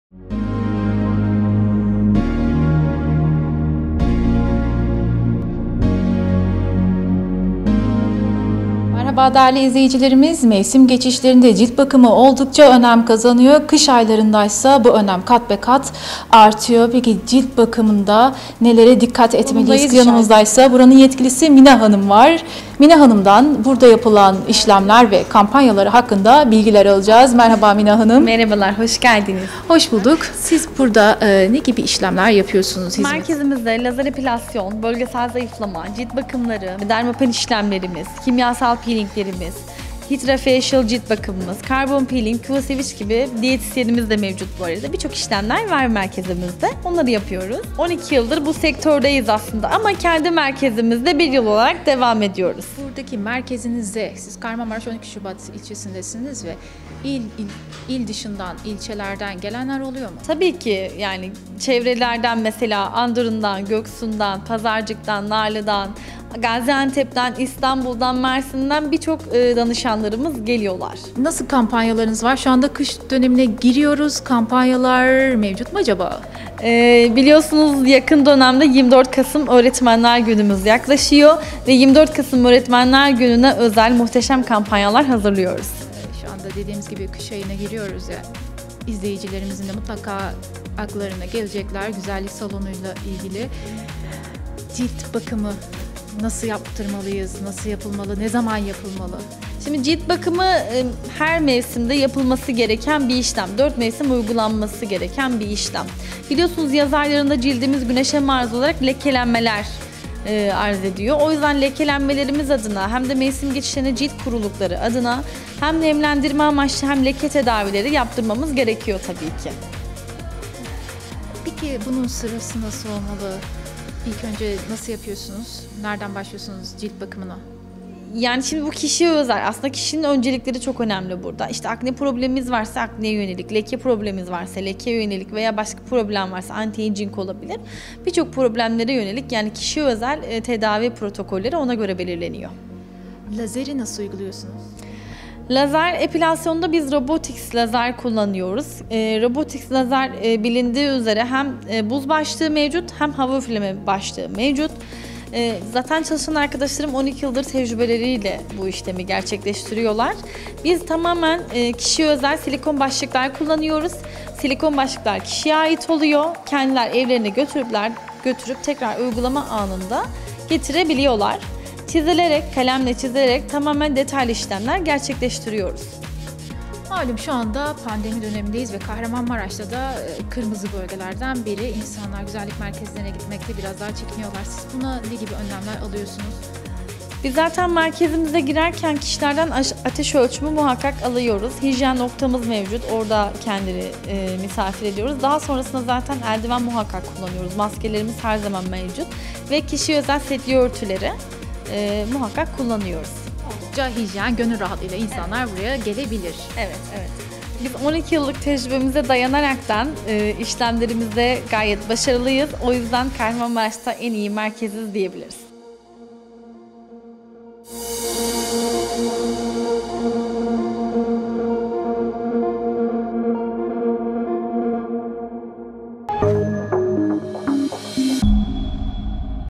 ESTETİK ANLAYIŞINA FARKLI BAKIŞ: MMARASH
Kahramanmaraş'ta hizmet sunan MMARASH Estetik & Güzellik, cildi rahatlatmak, G5 Masaj Uygulaması sayesinde kolayca zayıflama, lazer ile Karbon Peeling işlemiyle ciltte görülen sivilceler, lekeler gibi sorunlar ortadan kaldırarak ve daha birçok hizmetle, büyük bir titizlikle çalışıyor.